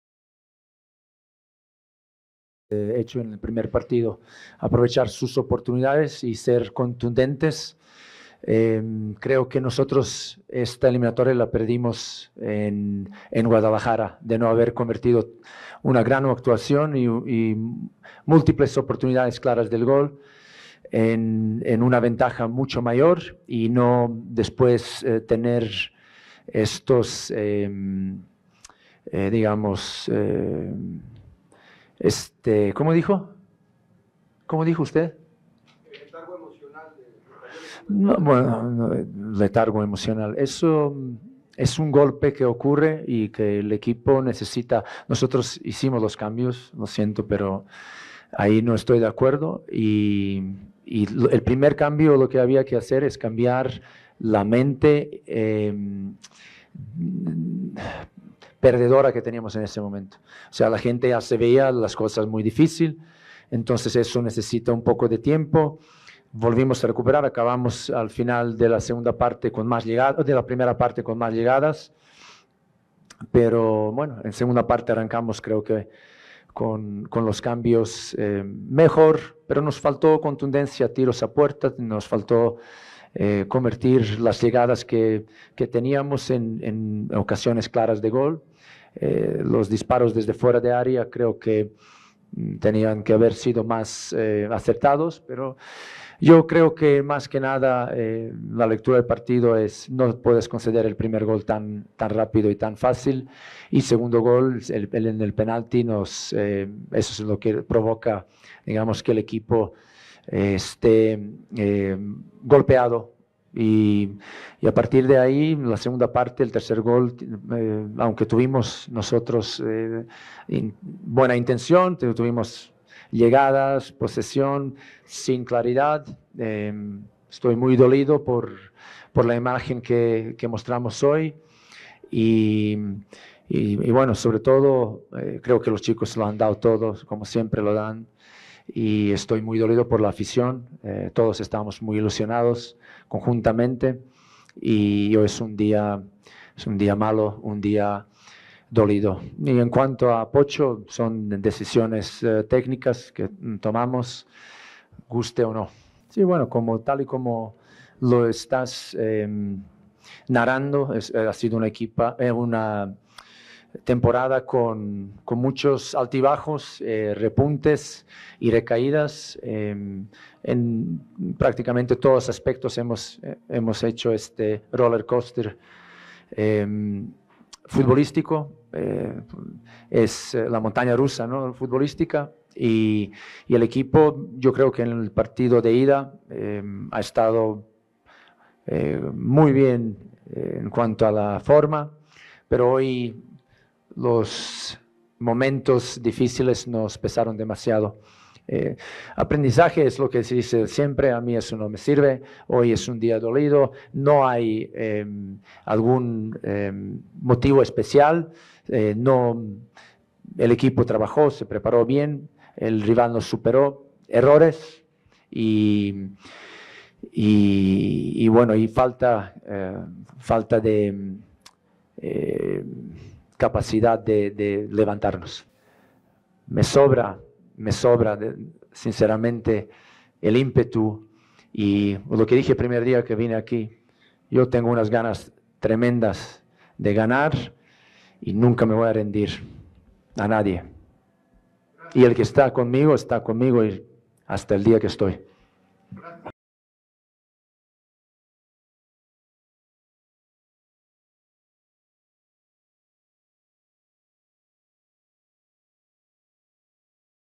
Paunovic: "Quedamos fuera en la ida"
Conferencia de prensa con el D.T. del Club Guadalajara tras su derrota y eliminación de cuartos de final de la Liga MX ante su similar del Club Universidad Nacional, comentó. Para más información visita